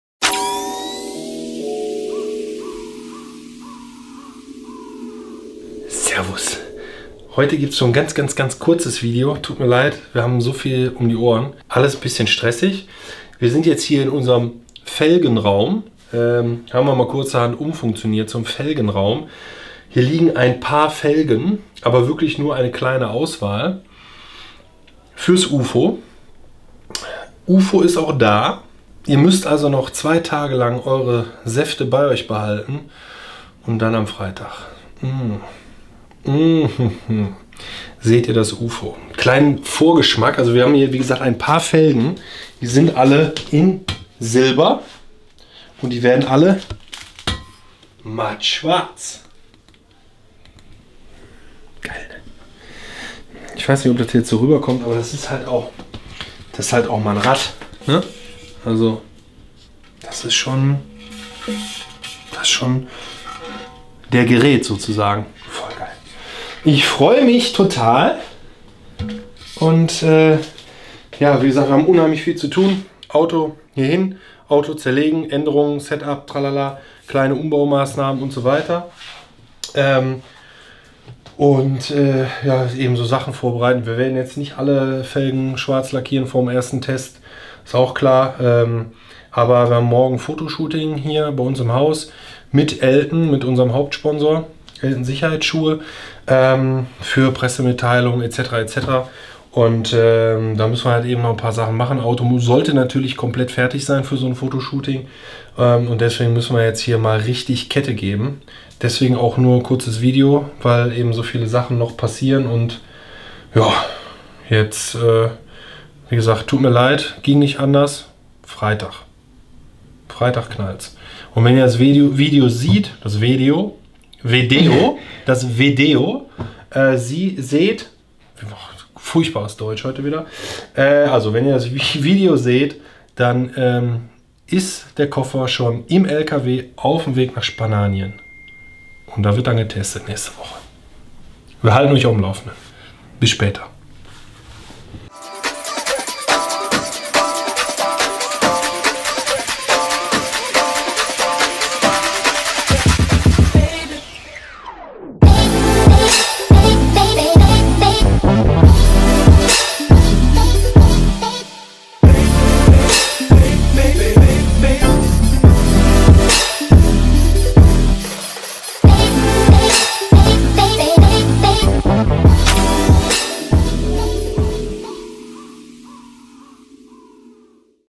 Er kommt!
Bald hat das Warten ein Ende. In zwei Tagen ist es soweit, endlich können wir euch unseren Rennwagen für die kommende Saison vorstellen. Doch bis dahin wartet noch etwas Arbeit auf uns.